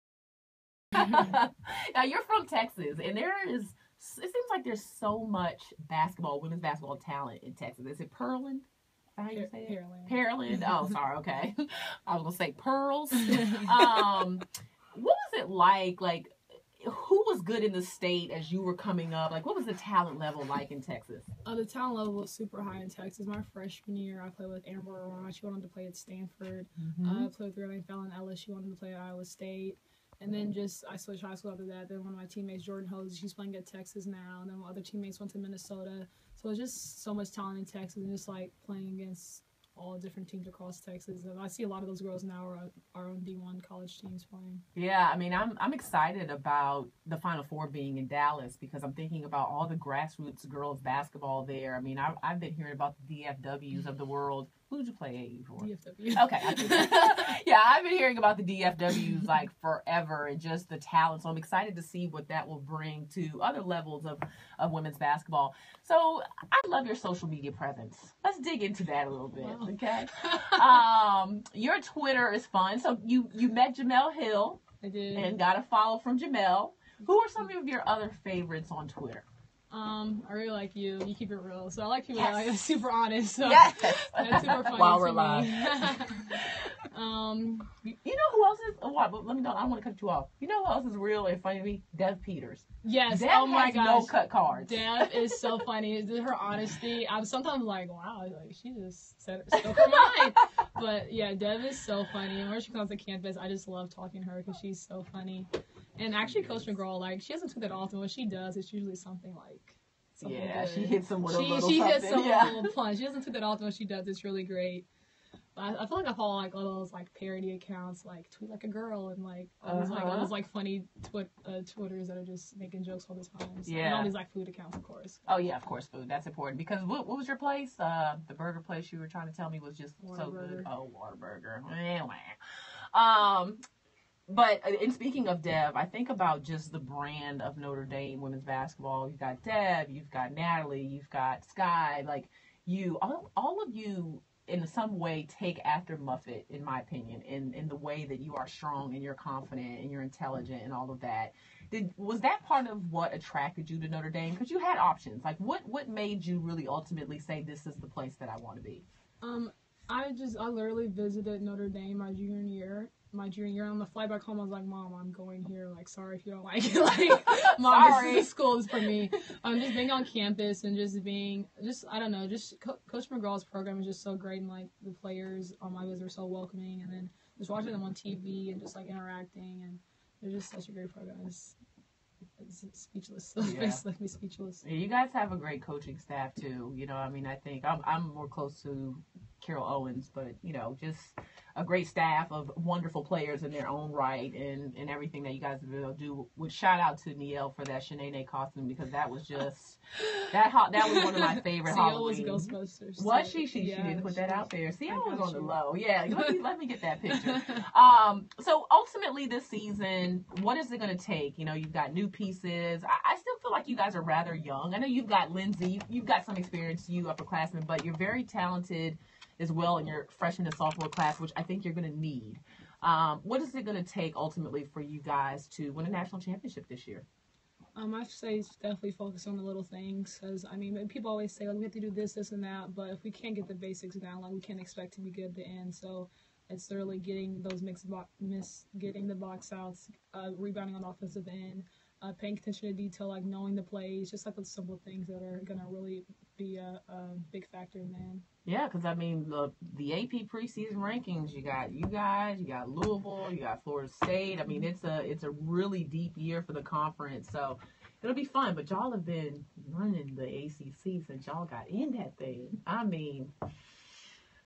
01/11/2016 English Hey @ndwbb AroundTheRim @LaChinaRobinson is live with Briana Turner for
On Periscope
-- Hey @ndwbb AroundTheRim @LaChinaRobinson is live with Briana Turner for NCAAW Media Day